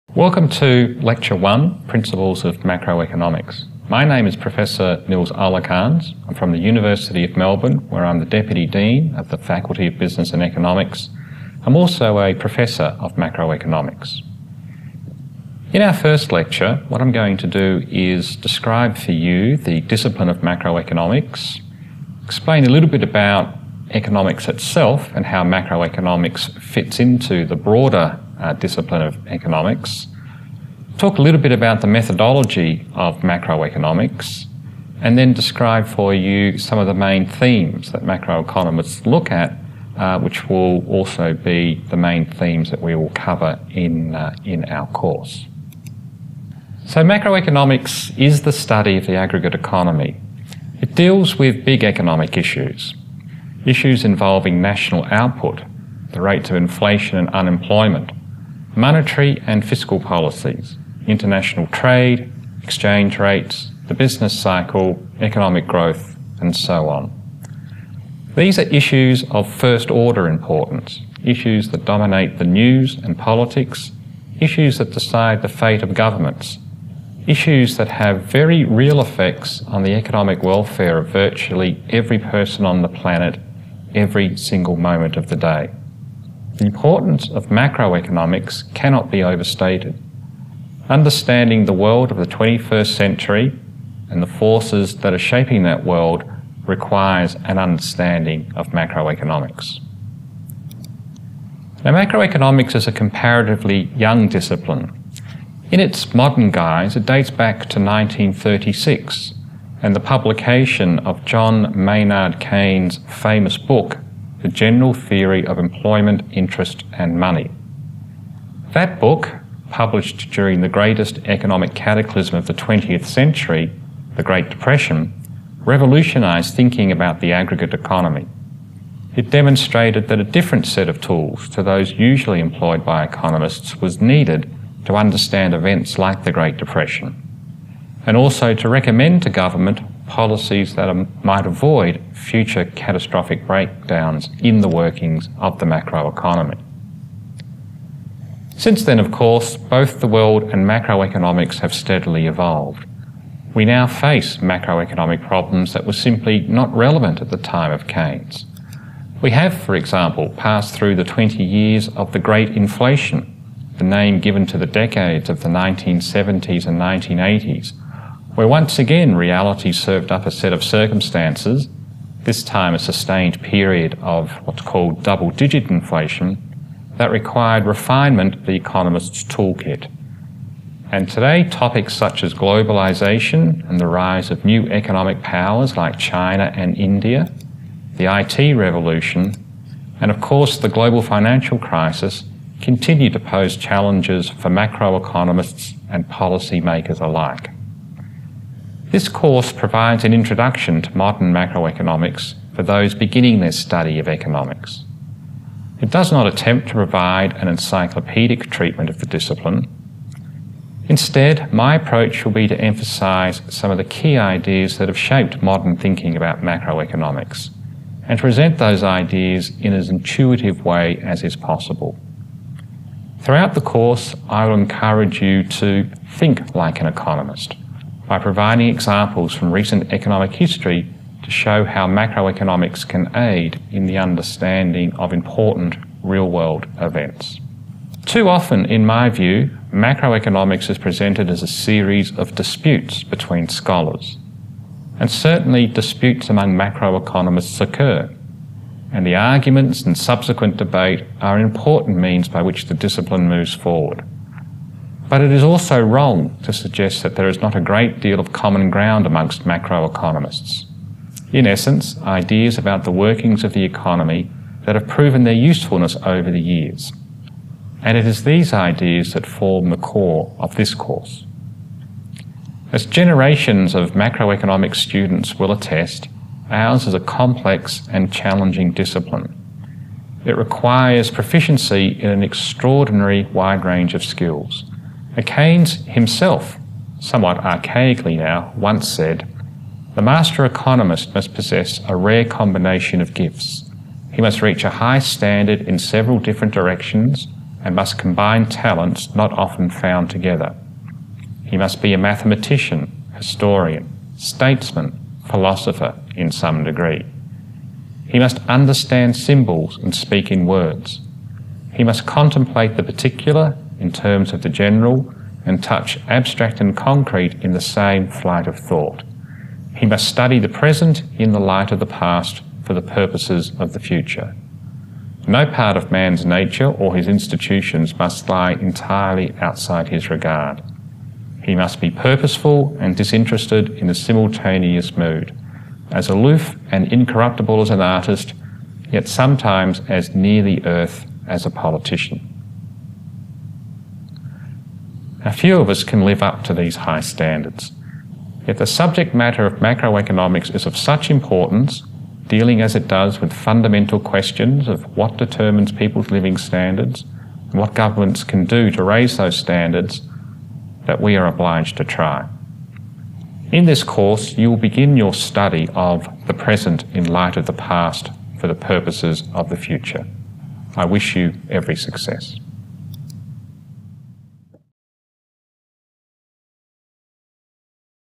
Module One - Macro Lecture 1 Part 1
Macro Lecture 1 Part 1

Principles of Macroeconomics by Nilss Olekalns (The University of Melbourne)

"In the first lecture we focus on introducing the concept of economics to you. We try to demystify this broad discipline down to one fundamental idea: economics is about decision-making."